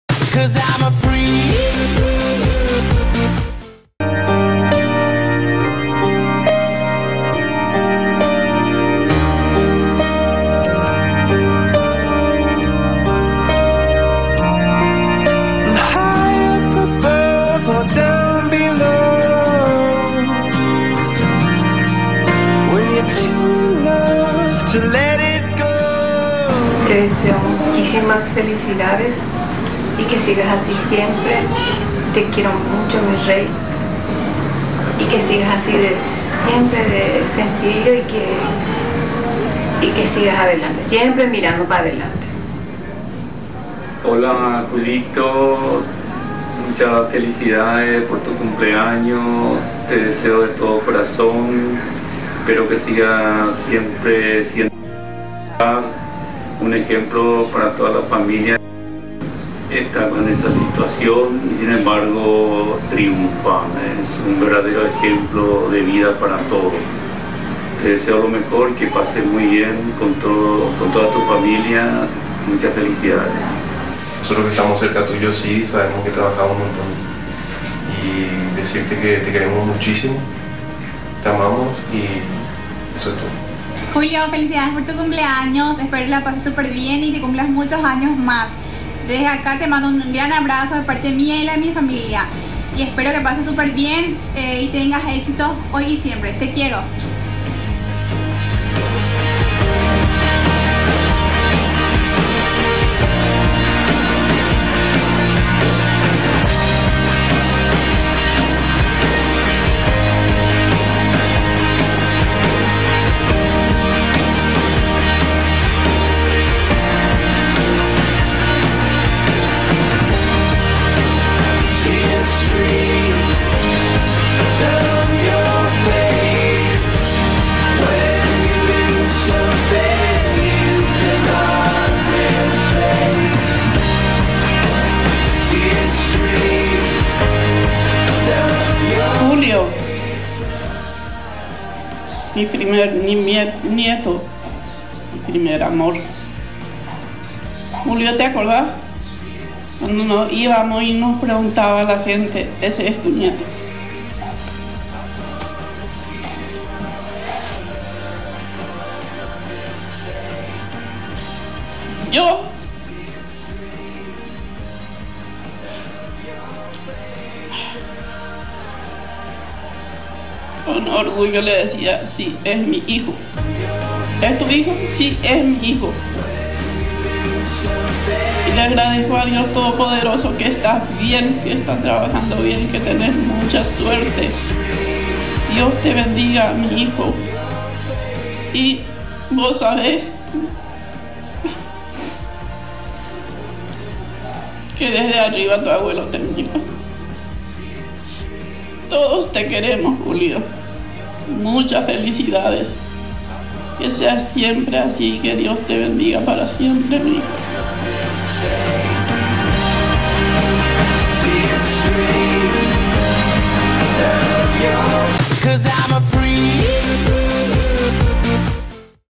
¡Feliz cumpleaños Mortero Querido!
Esta es la pequeña dedicatoria que le hicimos a nuestro compañero y estimado Julito, mas conocido como el gran "mortero bala" :) ¡Por muchos años mas de vida IDOLO!